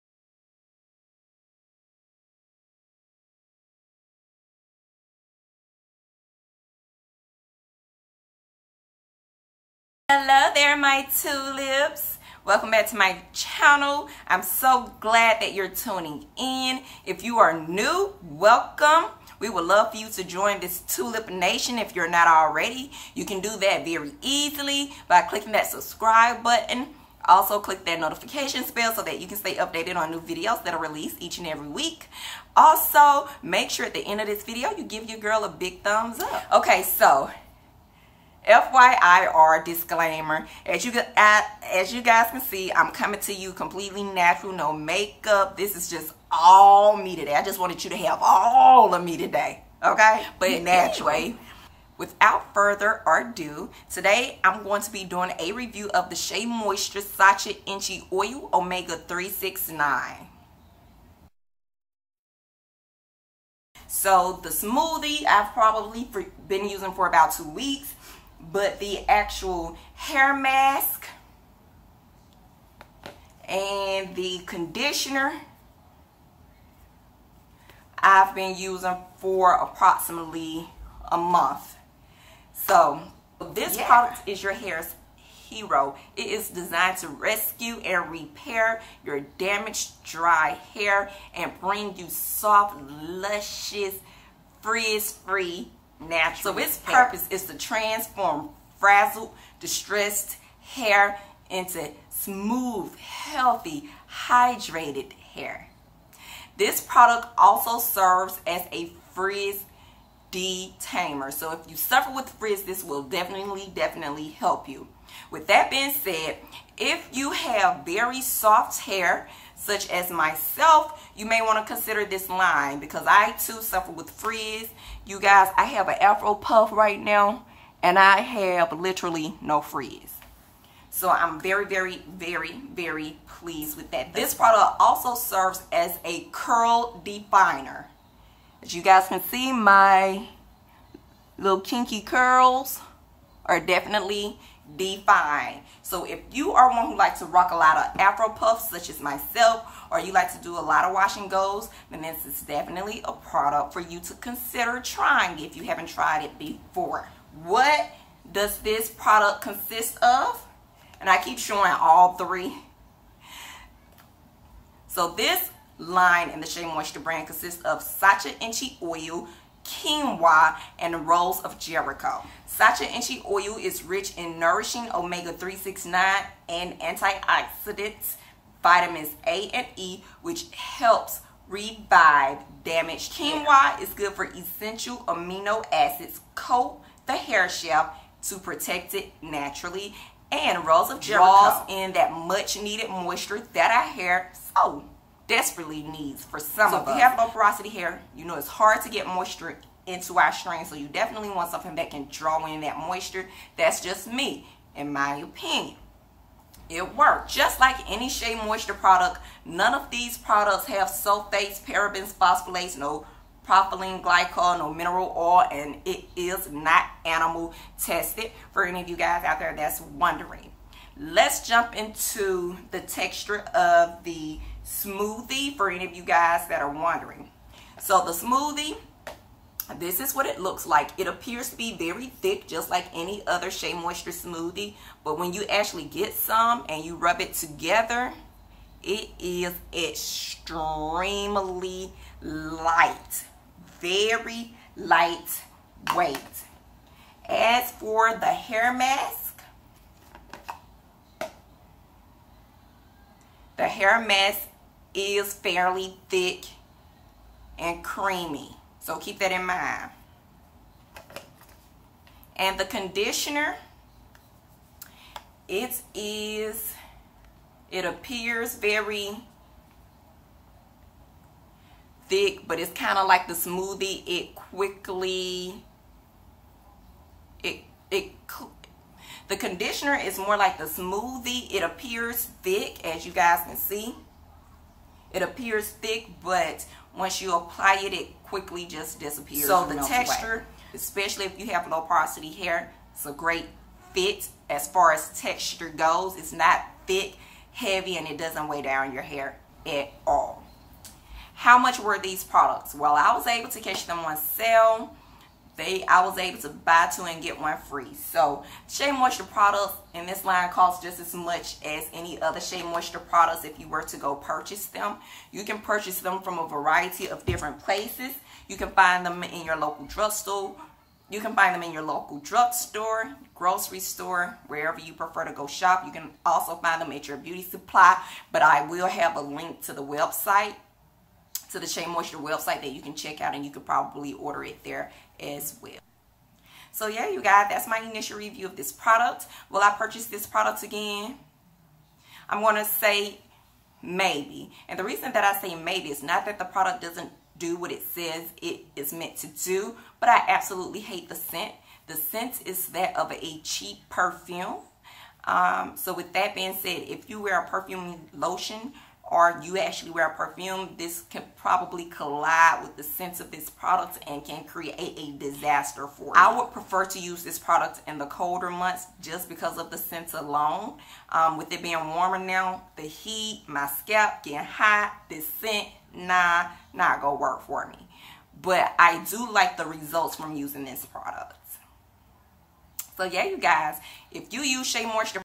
SHEA MOISTURE'S OMEGA 369 RESCUE AND REPAIR, INITIAL REVIEW
GREAT PRODUCT!!! I THINK THIS STUFF IS OKAY. YOU MAY THINK SO TOO!!!